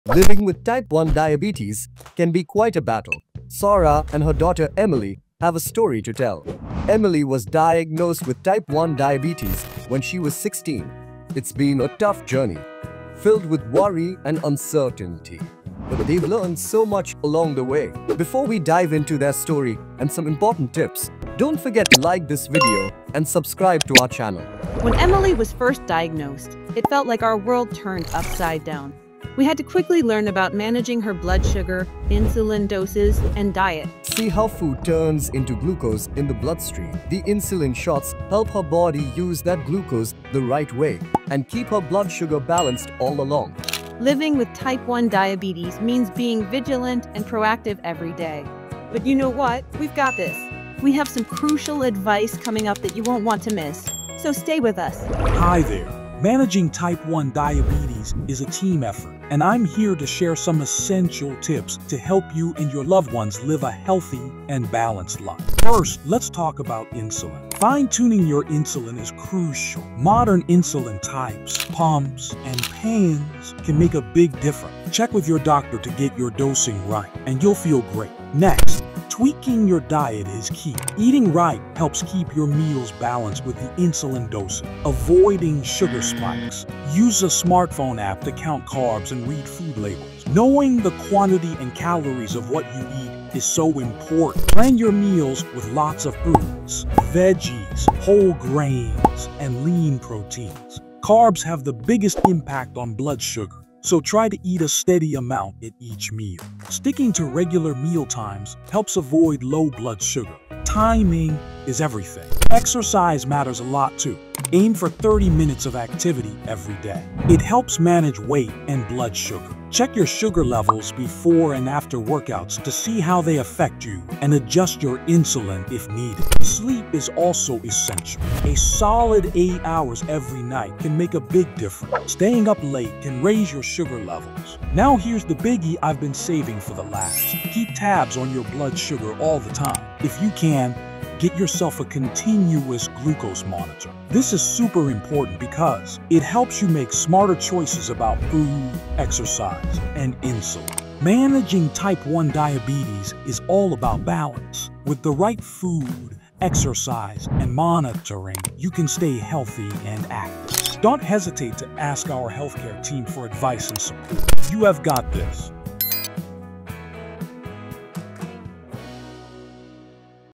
Winning over Type 1 Diabetes!
Are you or a loved one battling Type 1 Diabetes? Empower yourself with essential tips on balancing insulin, diet, and lifestyle. Keep watching and take control of your diabetes journey!